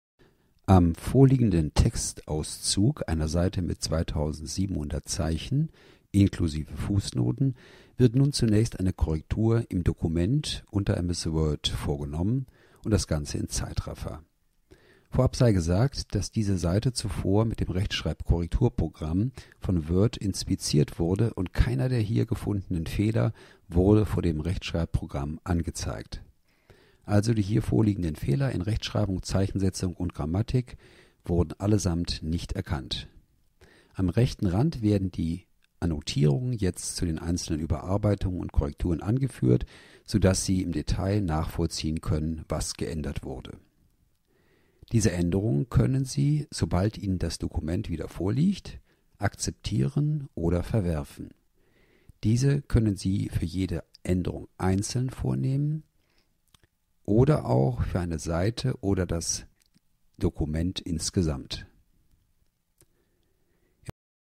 Korrektur und Lektorat im Dokument (MS Word) - Teil 1
Im Teil 1 wird gezeigt, wie eine Textseite am Bildschirm korrigiert und lektoriert wird.